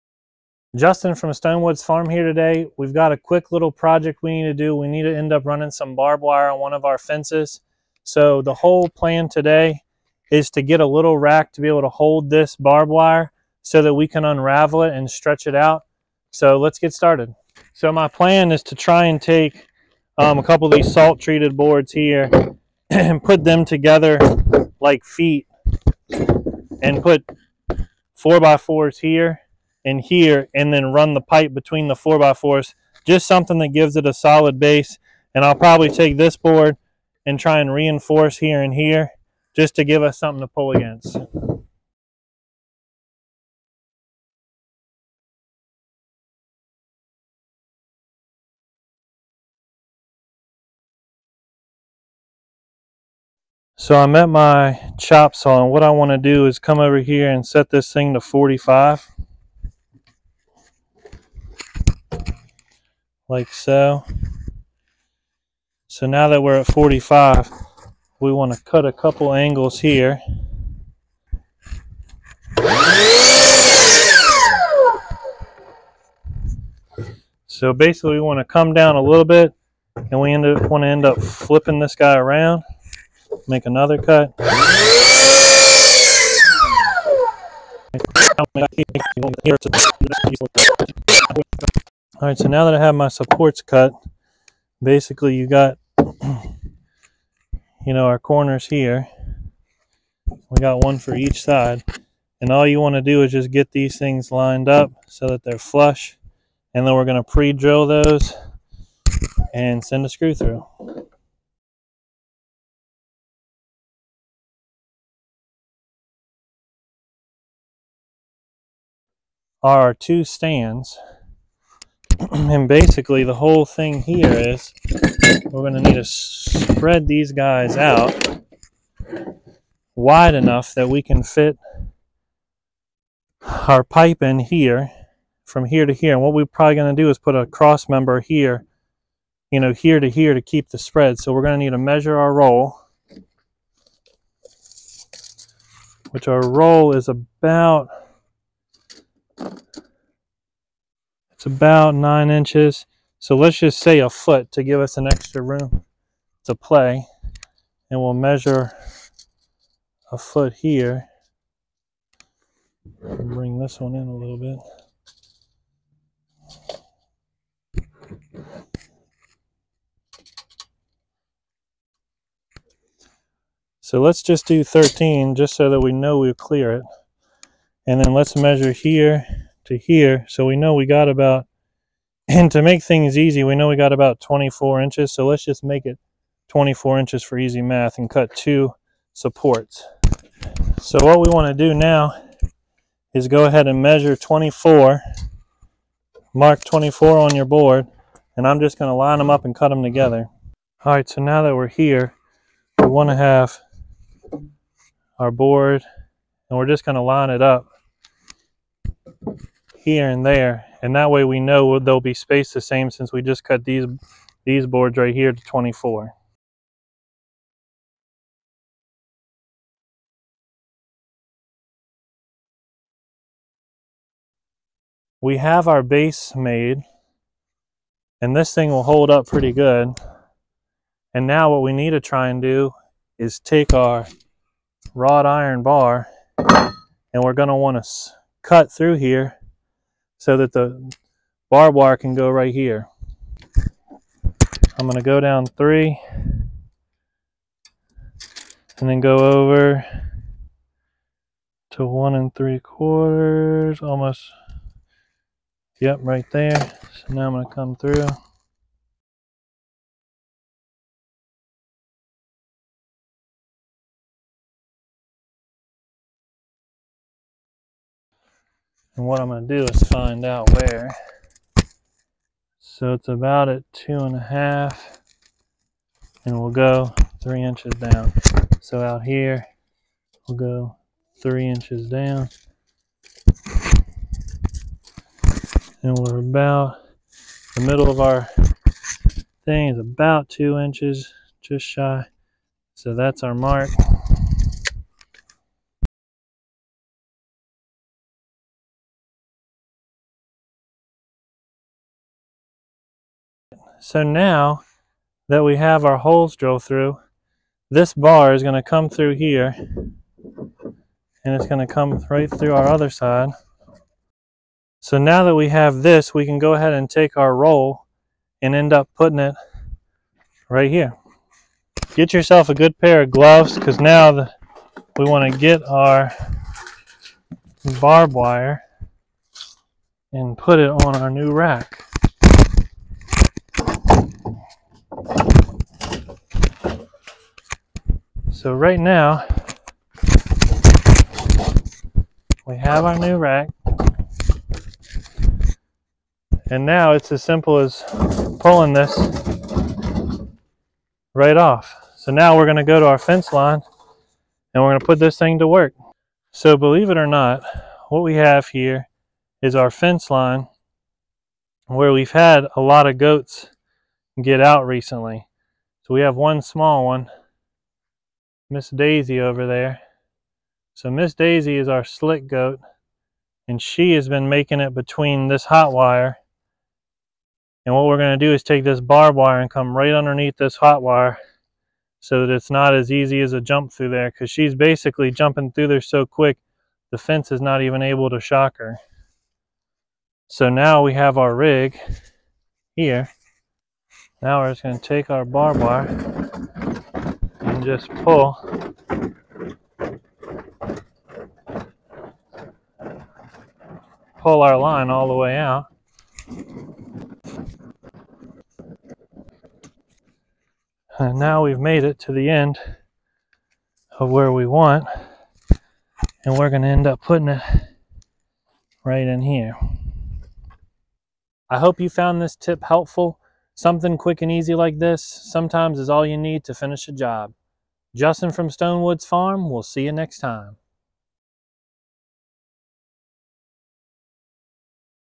Barb Wire fencing made easier!
Barb wire is always such a pain to deal with. This rack makes life a little easier and safer using a few scrap pieces around the farm.